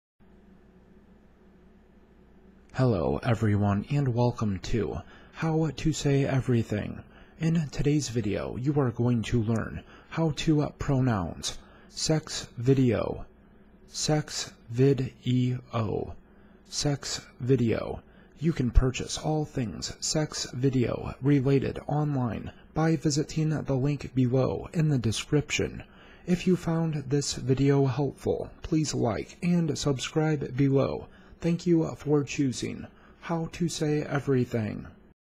How To Pronounce Sex Video
In this video you will learn how to pronounce this word: Sex Video
-Here's is the link mentioned in the video video&index=aps&camp=1789&creative=9325&linkCode=ur2&linkId=d807bd9e97db07c52252f1cd0db575b1
Also be sure to like this video as well as any other videos that you find to be helpful. Thank you for choosing how to say everything!